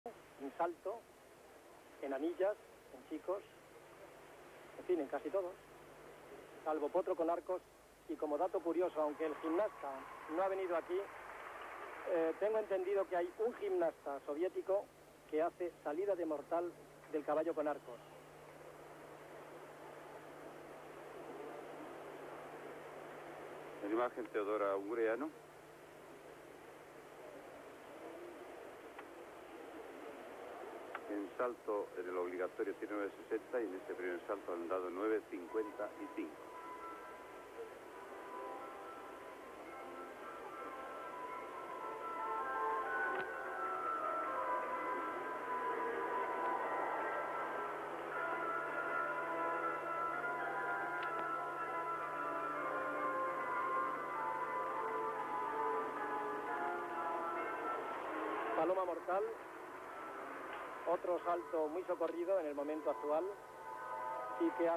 Teodora Ungureanu VT 1978 Gymnastics World Champs. team optionals
Romanian gymnast Teodora Ungureanu performs on vault at the 1978 World Championships.